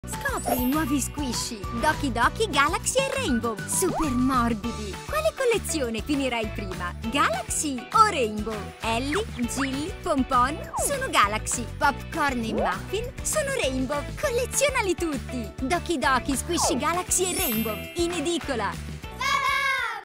Supermorbidi: sono arrivati gli Squishy Galaxy e Rainbow! 🤩
Doki Doki Squishy Galaxy e Rainbow!
Sono super morbidi! Quale collezione finirai prima?
Elly la medusa, Jilly l'unicorno sulla luna, PonPon l'orsetto galattico sono galaxy, mentre Poppis, Pop Corn e Muffin sono rainbow!
Collezionali tutti!